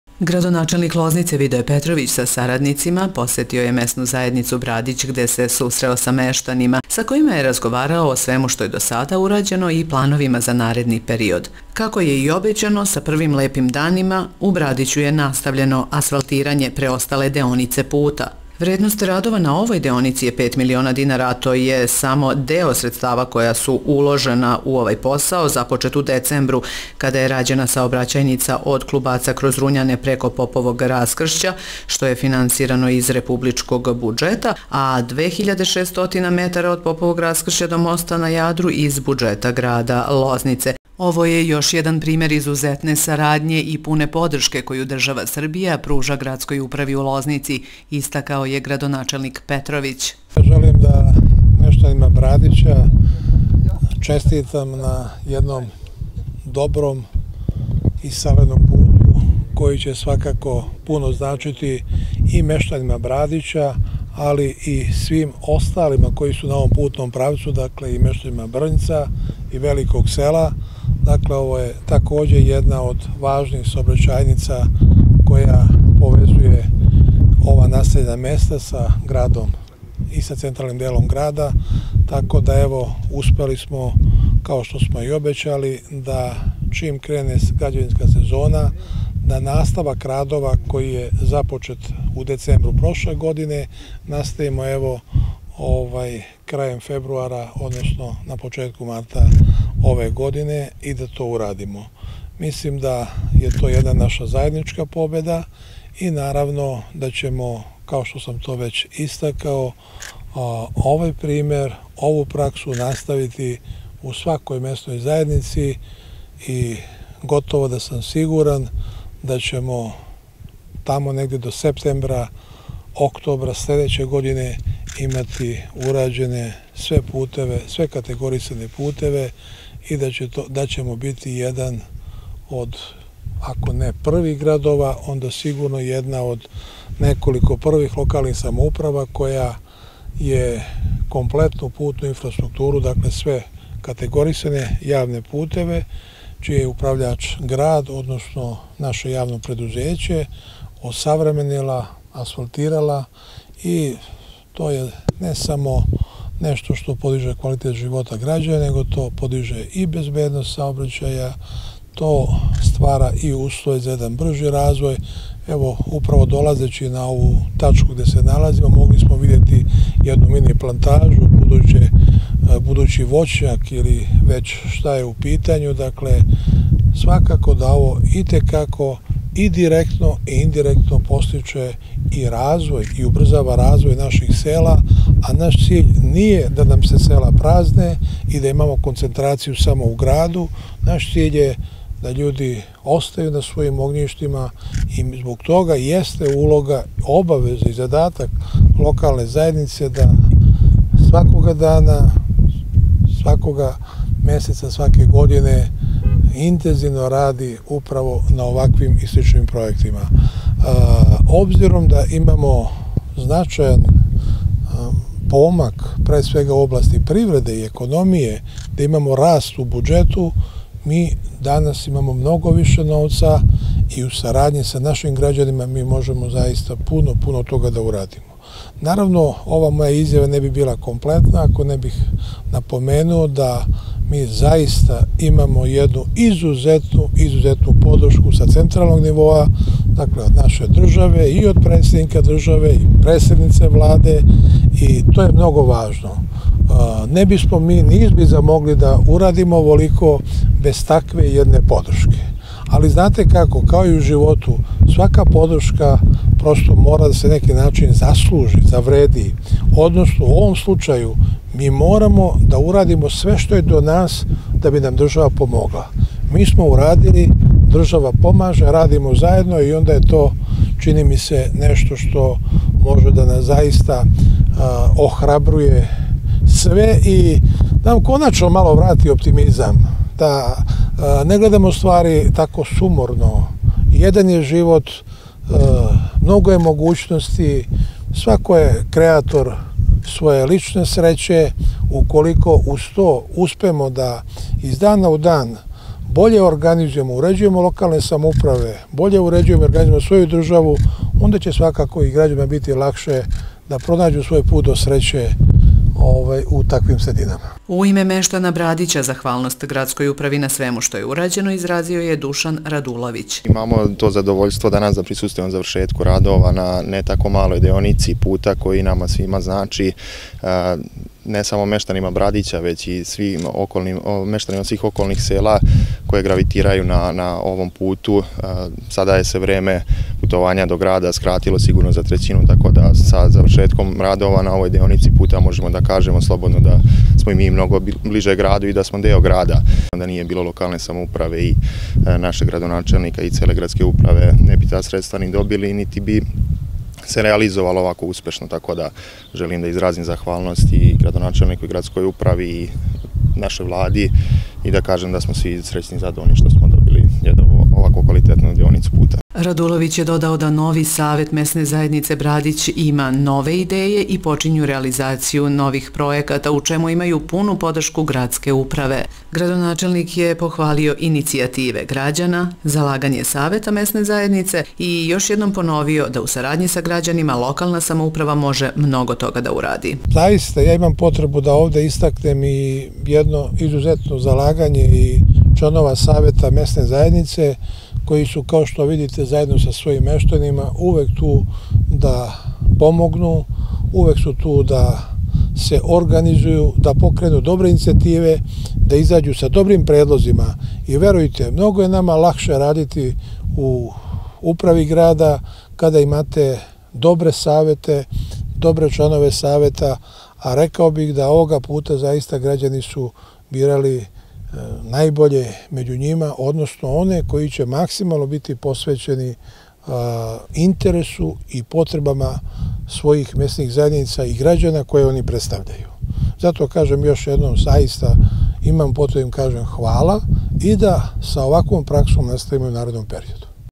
Равномеран развој свих лозничких месних заједница уз договор са грађанима
Градоначелник Лознице Видоје Петровић присуствовао је завршетку радова на асфалтирању 500 метара пута од Поповог раскршћа до моста у Брадићу где је у разговору са мештанима тог села сазнао шта су њихови приоритети и истакао да је  циљ локалне самоуправе да грађани остану на својим огњиштима и да се зато спроводе пројекти и интезивни радови који доприносе убрзаном развоју Лознице.

- С обзиром на то да имамо значајан помак у области привреде и економије, раст у буџету и много више средстава и у сарадњи са нашим грађанима можемо много тога да урадимо. Имамо изузетну подршку са централног нивоа наше државе, и од председника Србије и председнице Владе што је много важно, јер не бисмо све ово могли да урадимо без њих. Морамо да чинимо све што је до нас да би нам држава помогла. Уколико уз то успемо из дана у дан да боље организујемо и уређујемо локалне самоуправе и државу онда ће и грађанима бити лакше да пронађу свој пут до среће - указао је градоначелник Лознице и додао да ће нови асфалт пуно значити не само мештанима Брадића већ и становницима месних заједница Брњца и Великог Села, које су на овом путном правцу.Како је навео, ова, у потпуности реконструисана саобраћајница, укупне дужине 5.5 километара, води од Клубаца, кроз Руњане, преко Поповог раскршћа до Брадића и повезује насељена места са централним делом града.

- Ово је наша заједничка победа и овакву праксу наставићемо у свакој месној заједници и готово да сам сигуран да ћемо до септембра, октобра следеће године имати уређене све категорисане путеве и да ћемо бити један од, ако не првих градова онда једна од првих локалних самоуправа која је све категорисане јавне путеве, чији је управљач град, осавременила и асфалтирала што подиже не само квалитет живота грађана већ и безбедност у саобраћају и ствара услове за бржи развој - закључио је градоначелник.